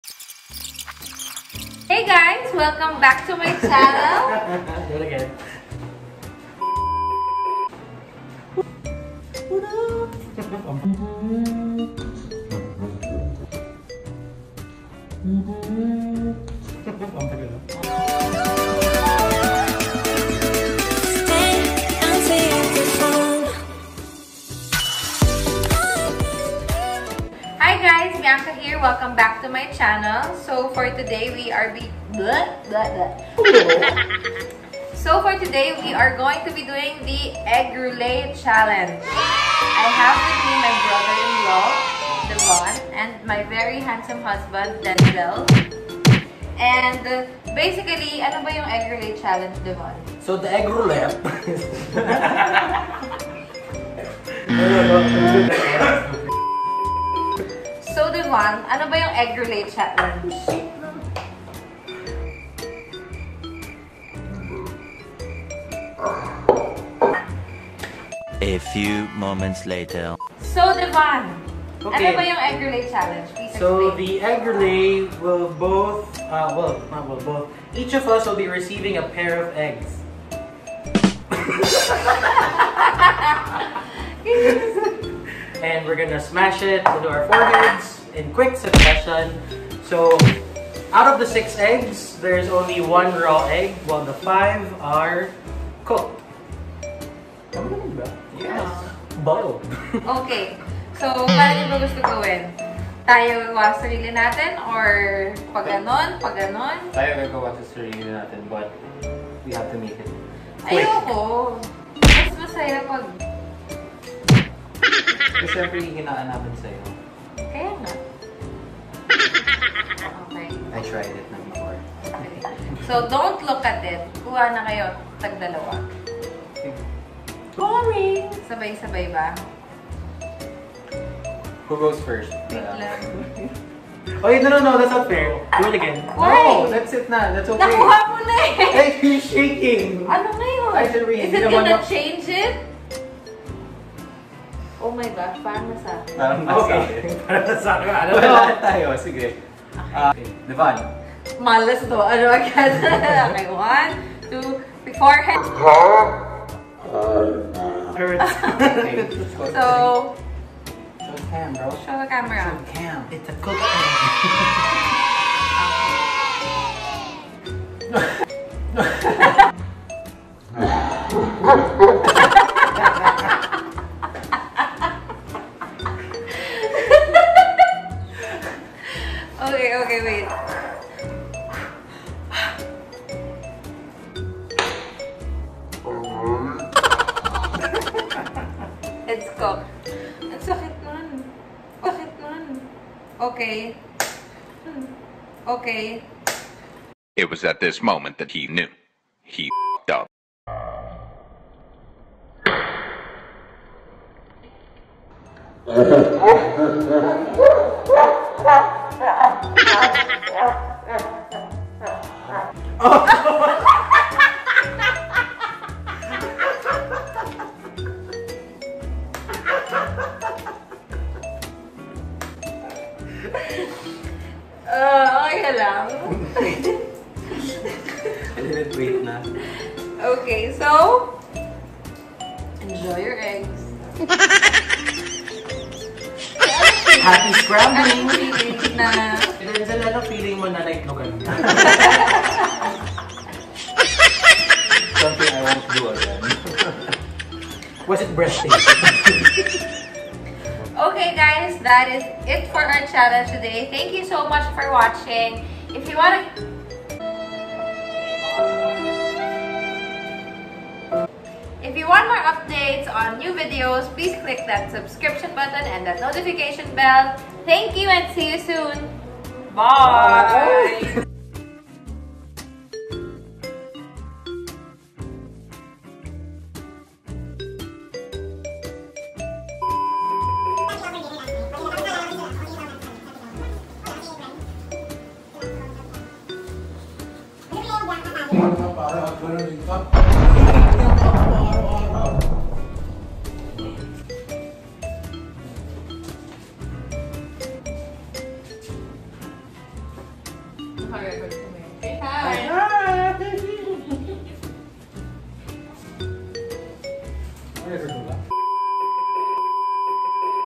EGG ROULETTE CHALLENGE
Doing the egg roulette challenge with my husband Denzel and brother in law, Devan.